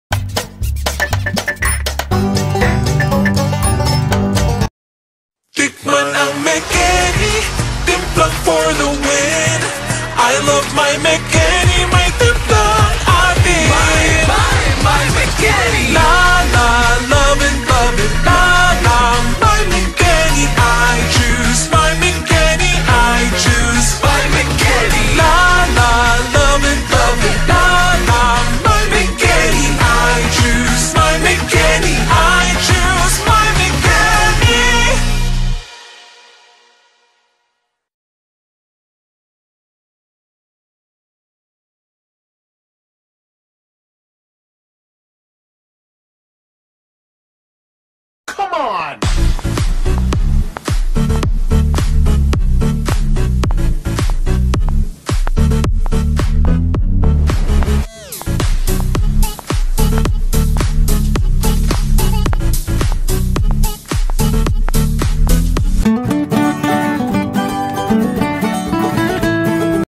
MEKENI | HAIER
A Blessed Day Everyone. For this Vlog just to show my TikTok entries for Mekeni FAcebook Page and Haier Facebook Page.

At gusto ko rin po ipaalam na nilagyan ko ng speed ang audio kaya kung mapapansin po ninyo nag iba po ang kinalabasan ng music.

Thanks and Godbless Us All!